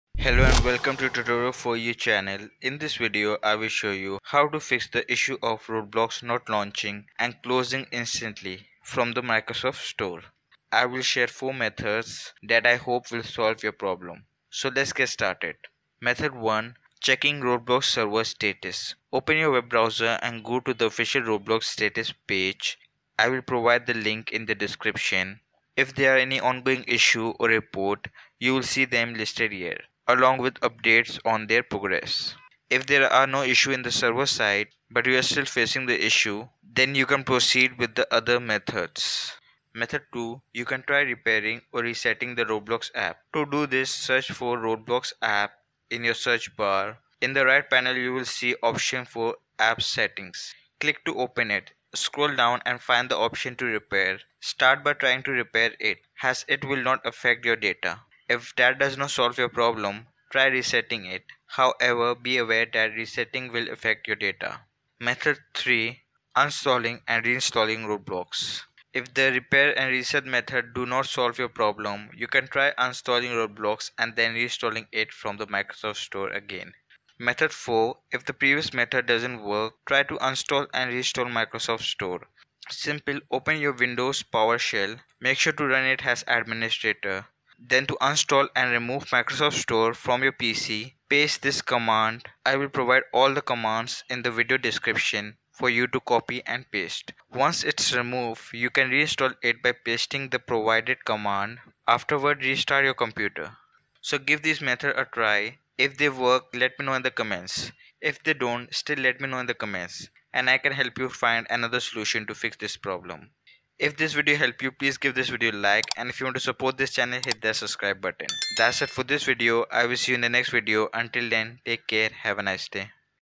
How to Fix Roblox Not Launching And Closes Instantly in Microsoft Store
Welcome to the Tutorial 4You Channel! Are you facing trouble with Roblox not launching and closing instantly when you open it from the Microsoft Store? Don't worry, we've got you covered! In this video, we'll walk you through four effective methods to troubleshoot and fix this annoying issue. Whether it's a server problem or an app-related glitch, we've got the solutions you need.

Method 1 involves checking the current Roblox server status to rule out any ongoing server issues. Find out how to do this step-by-step to ensure you're not experiencing problems beyond your control.

Method 2 guides you through repairing or resetting the Roblox app. Learn how to navigate app settings and apply these options correctly to potentially resolve the problem.

Method 3 covers the process of uninstalling and reinstalling Roblox from the Microsoft Store. Sometimes, a fresh installation can do wonders in fixing persistent issues.

Method 4 takes it a step further, explaining how to uninstall and reinstall the Microsoft Store itself if the previous methods haven't worked. Follow these instructions carefully for a comprehensive solution.
To Remove Microsoft Store
a)Get-AppxPackage *windowsstore* | Remove-AppxPackage
To Reinstall Microsoft Store
b)Get-AppxPackage -allusers Microsoft.WindowsStore | Foreach {Add-AppxPackage -DisableDevelopmentMode -Register "$($_.InstallLocation)\AppXManifest.xml"}